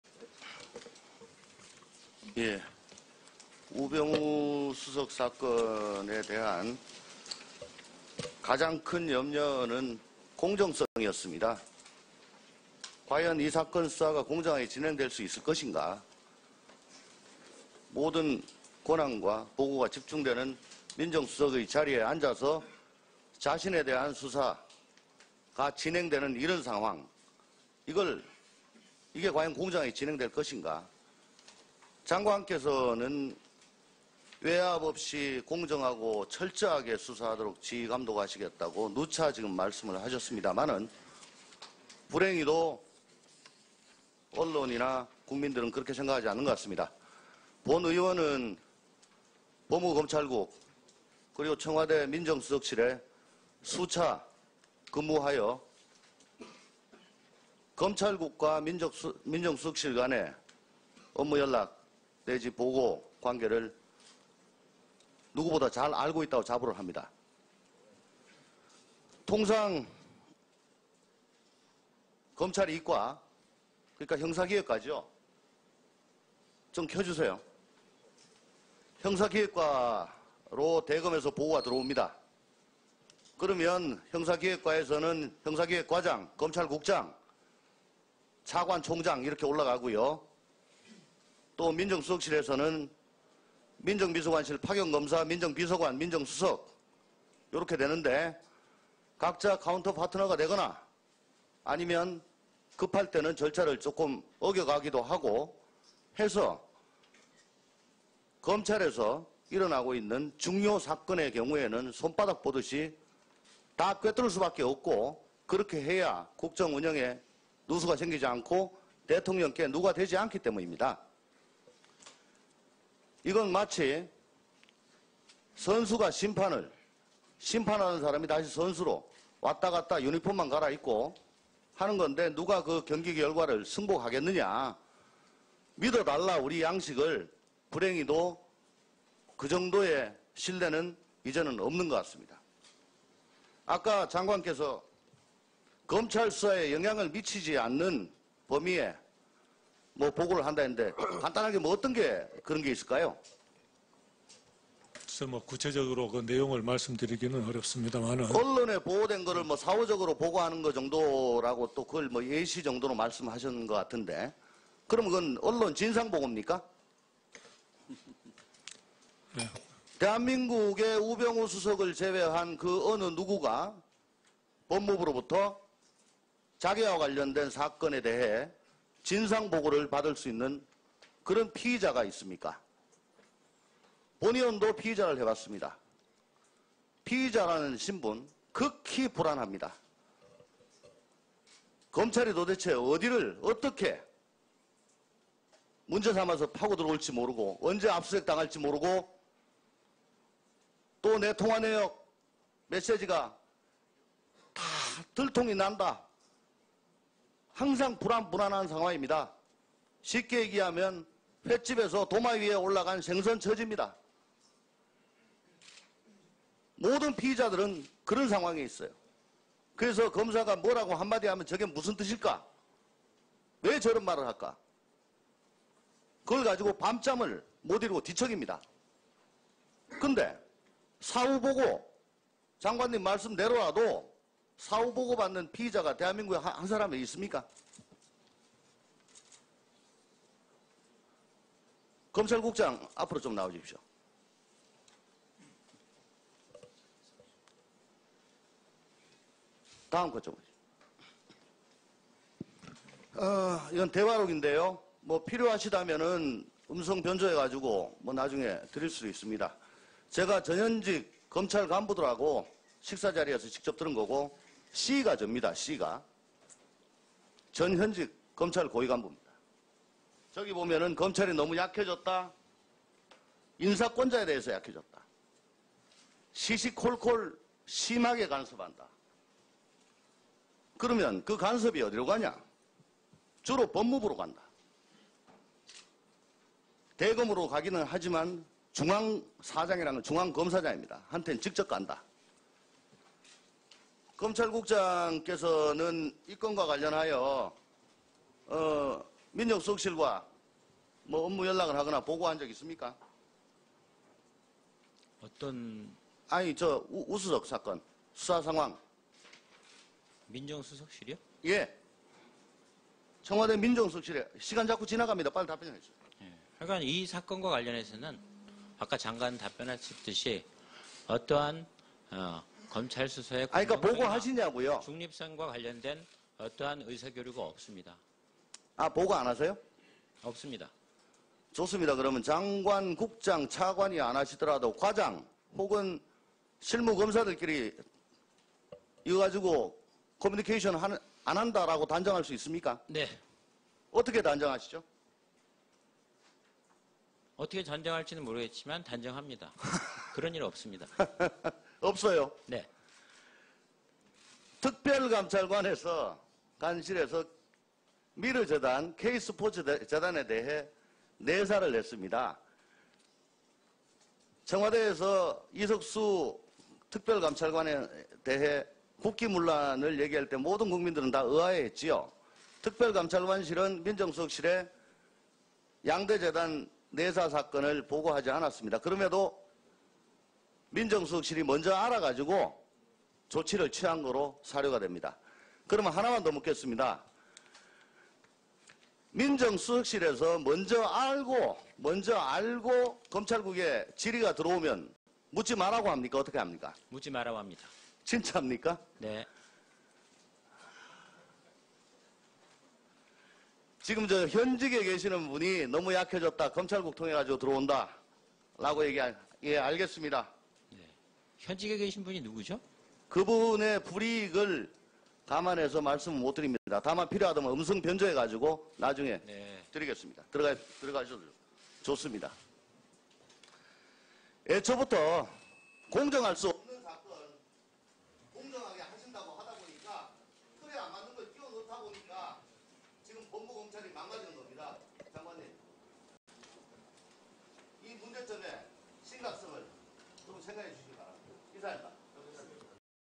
조응천 의원-민정수석실에서 검찰 주요사건 꽤 뚫고 있다" 전 현직 고위검찰 녹취록 공개-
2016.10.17 법무부 국정감사 조응천 전 공직기강 비서관 질의 우병우 사건이 어떻게 공정하게 진행될수 있나...검찰의 주요사건들이 민정수석실로 보고 되는 상황에서 청와대 근무해봐서 잘 안다...특별감찰관 이석수 미르재단 k스포츠 내사중 민정수석실이 더 먼저 알고 있었다... 전 현직 검찰 고위 간부 녹취록 공개...